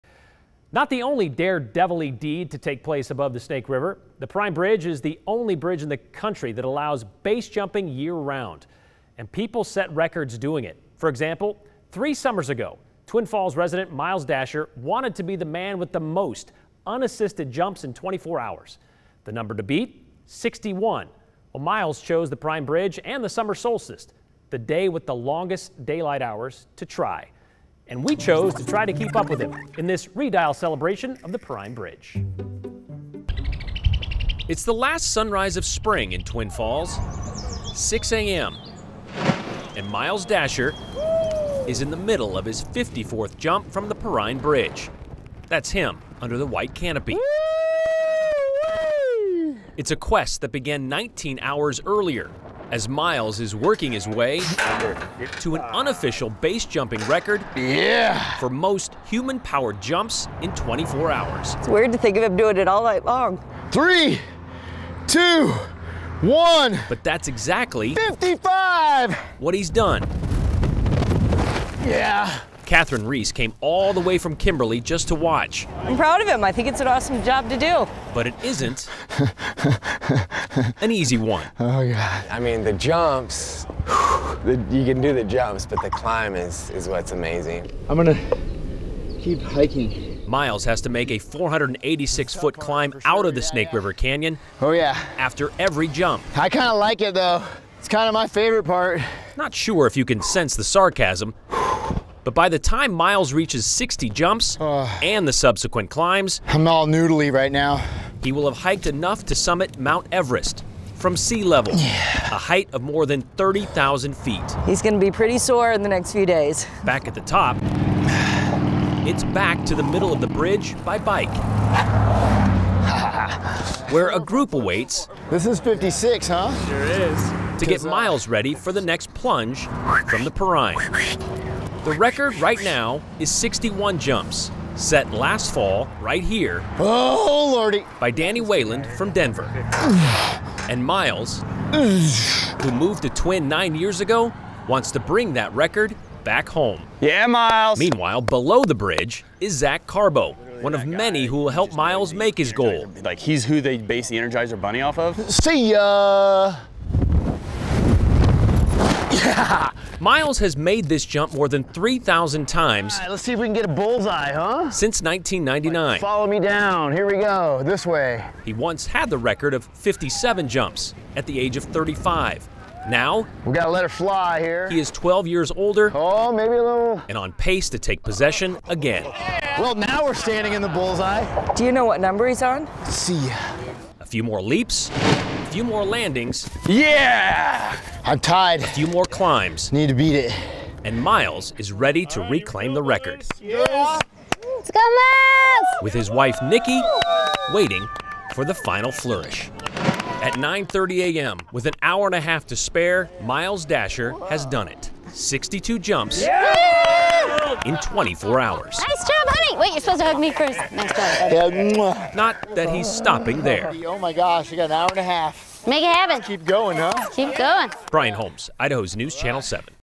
208 Redial: Twin Falls BASE jumper breaks world record
Over the course of a grueling 24 hours in 2017, Miles Daisher BASE jumped from Perrine Bridge 62 times, breaking the previous record.